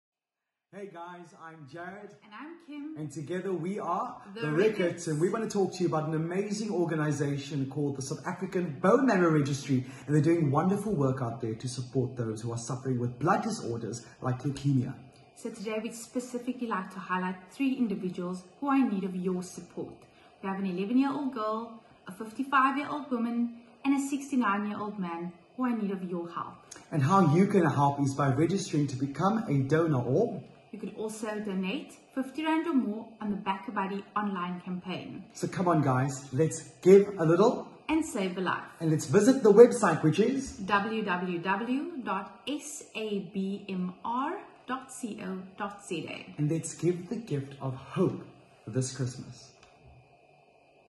Give A Little Save A Life
This festive season many families will find themselves gathered around a hospital bed of a loved one diagnosed with a life-threatening blood disorder in need of a stem cell transplant, instead of the Christmas table.

To ease the financial burden of these families, the South African Bone Marrow Registry (SABMR) has again launched their 'Give A Little Save A Life' campaign, with the aim of raising R300 000 for their Patient Assisstance Programme this year. These funds will be used to cover the cost of donor rectuitment, donor searches and related medical bills.